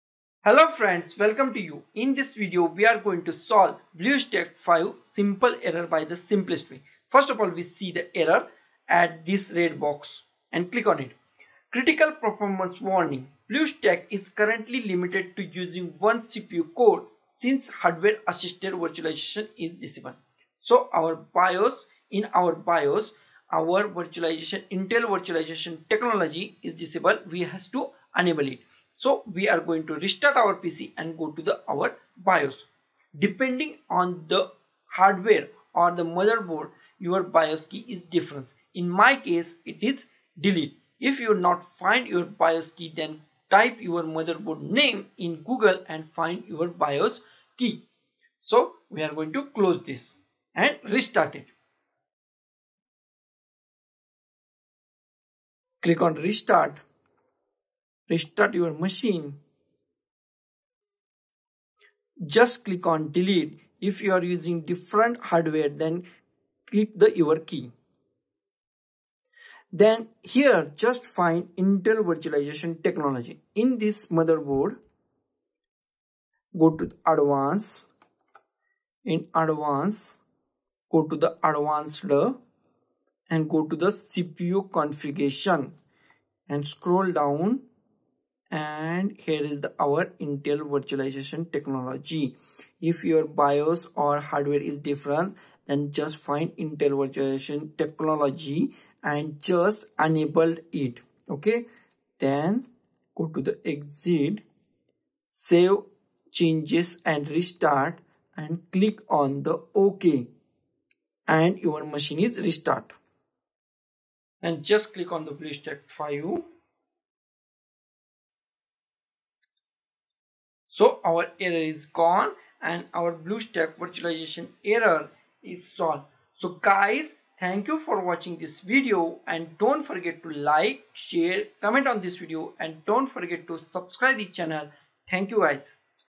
How to Fix Bluestacks 5 Performance Warning virtualization is disabled
Critical performance warning BlueStacks is currently limited to using 1 CPU core since Hardware - assisted virtualization is disabled  This warning is showing in our BlueStacks 5 And we are going to solve this problem by the very simplest way.   first we see the problem the problem is our  Intel virtualization technology is disabled in our BIOS so we want to enable it in bios and our problem is solved.

Here's a list of common BIOS keys by brand. Depending on the age of your model, the key may be different.

ASUS: F2 for all PCs, F2 or DEL for Motherboards
Acer: F2 or DEL
ASRock: F2 or DEL
ECS: DEL
Dell: F2 or F12
HP: F10
Gigabyte / Aorus: F2 or DEL
Lenovo (Consumer Laptops): F2 or Fn + F2
Lenovo (ThinkPads): Enter then F1.
Lenovo (Desktops): F1
Microsoft Surface Tablets: Press and hold volume up button.
MSI: DEL for motherboards and PCs
Origin PC: F2
Toshiba: F2
Samsung: F2
Zotac: DEL